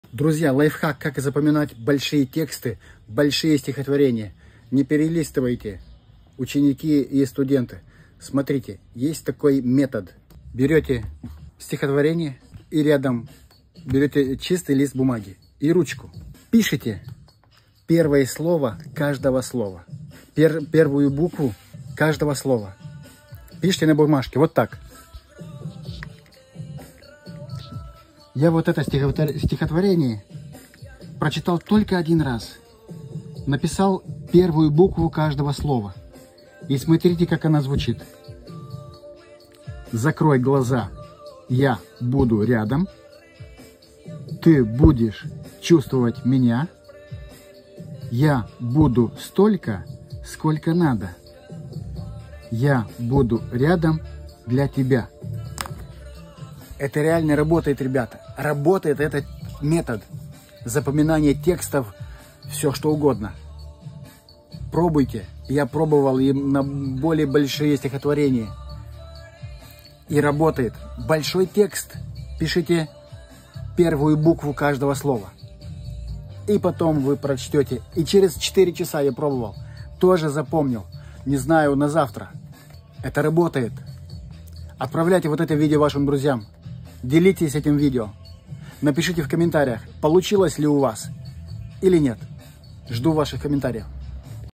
Как быстро запомнить стихотворение?
Метод как быстро запомнить большой текст или стих.?